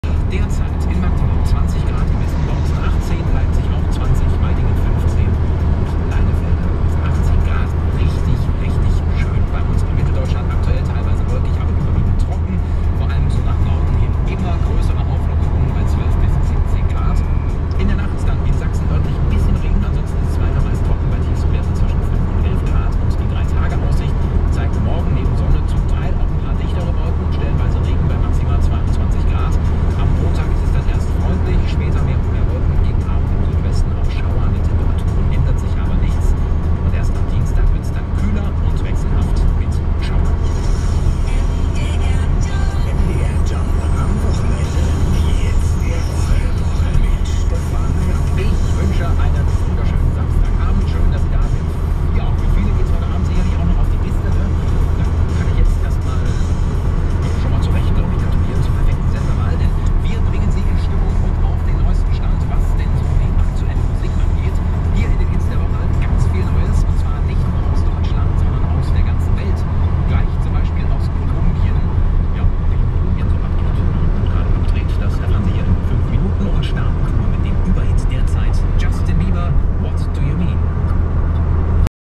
Autobahn Driving
Driving on the Autobahn in Germany and getting passed by a driver in a much bigger hurry than myself.